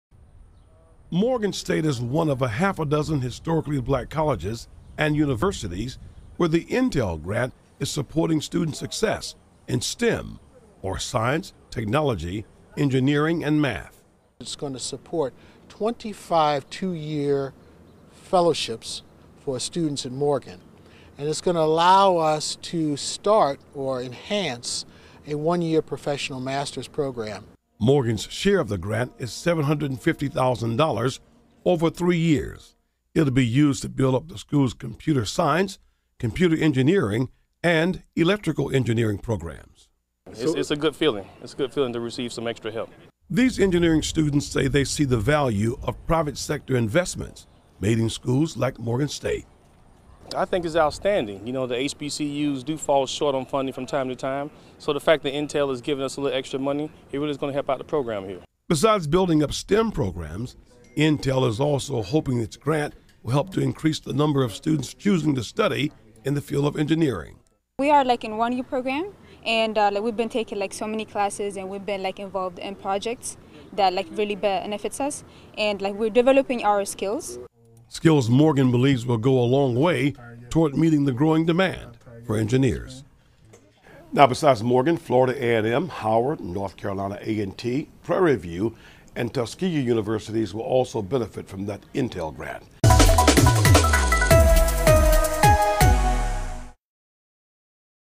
Video: Morgan State gets big boost from Intel
Morgan State University's engineering program will share part of a $4.5 million grant from technology giant Intel. Morgan State is one of a half-dozen historically black colleges and universities where the Intel grant is supporting student success in science, technology, engineering and math.